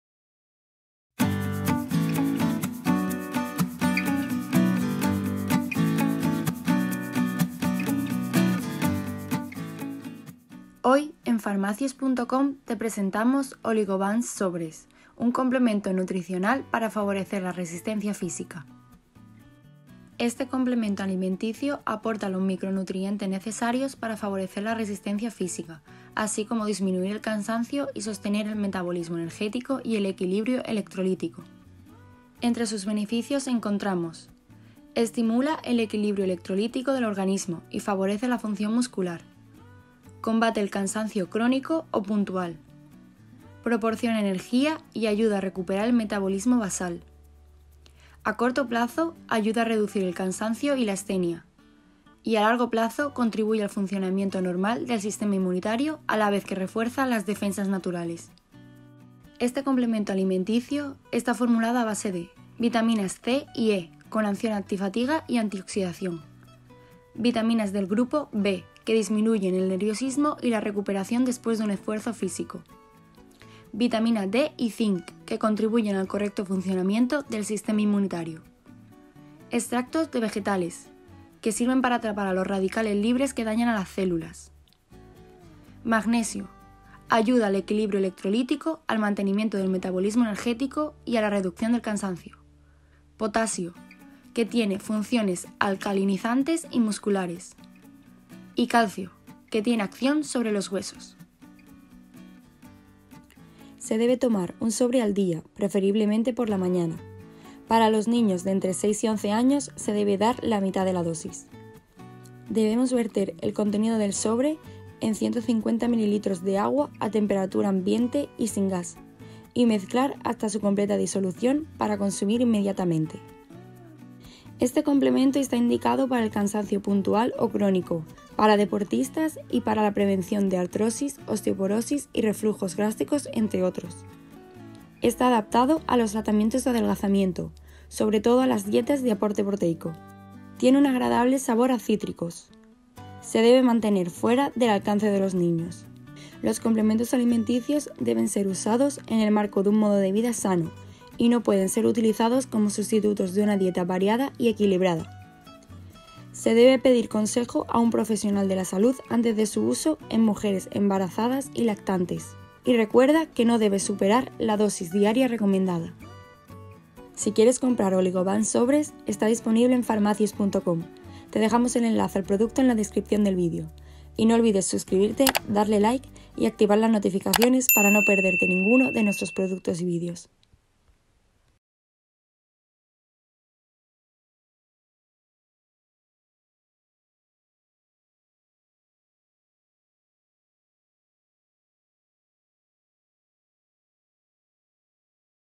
OLIGOVANCE SOBRES de INOVANCE: Composición, Para qué sirve..
00:00 Intro
00:10 Presentación
00:19 ¿Para qué sirve?
00:38 Beneficios
01:00 Composición
01:42 ¿Cómo tomar?
02:04 Indicaciones
02:47 Comprar OLIGOVANCE en pharmacius.com

¡Comparte tu opinión sobre OLIGOVANCE!